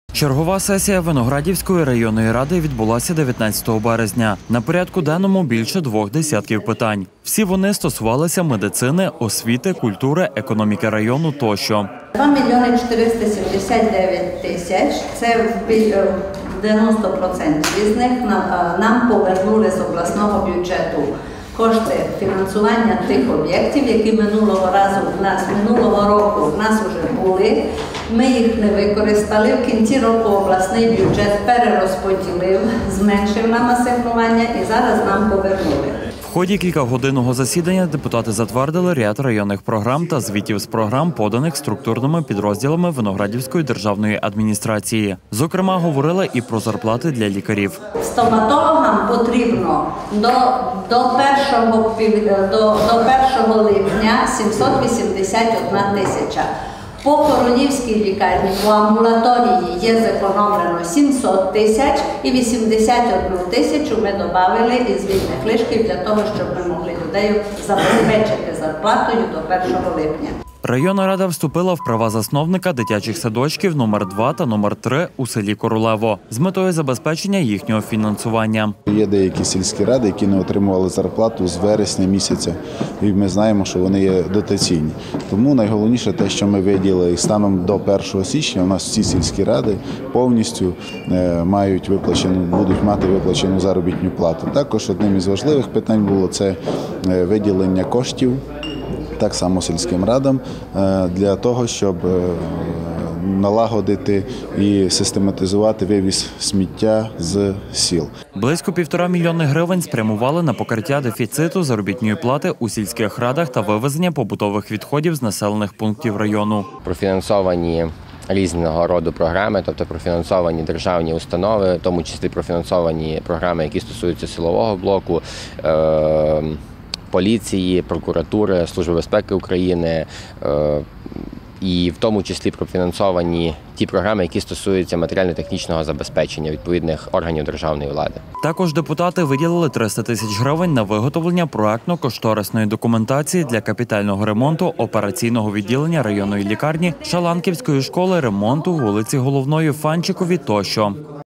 Ухвалені рішення
Чергова сесія Виноградівської районної ради відбулася, 19 березня. На порядку денному більше 2 десятків питань, всі вони стосувалися медицини, освіти, культури, економіки району, тощо. В ході кілька годинного засідання депутати затвердили ряд районних програм та звітів з програм, поданих структурними підрозділами Виноградівської державної адміністрації. Зокрема говорили і про зарплати для лікарів. В ході кілька годинного засідання депутати затвердили ряд районних програм та звітів з програм, поданих структурними підрозділами Виноградівської державної адміністрації. Зокрема говорили і про зарплати для лікарів. Районна рада вступила в права засновника дитячих садочків №2 і №3 у селищі Королево, з метою забезпечення їхнього фінансування. Близько 1,5 млн гривень спрямували на покриття дефіциту заробітної платі у сільських радах та вивезення побутових відходів з населених пунктів району. Також депутати виділили 300 тисяч грн на виготовлення проектно-кошторисної документації для капремонту операційного відділення районної лікарні, Шаланківської школи, ремонт вулиці Головної в Фанчикові.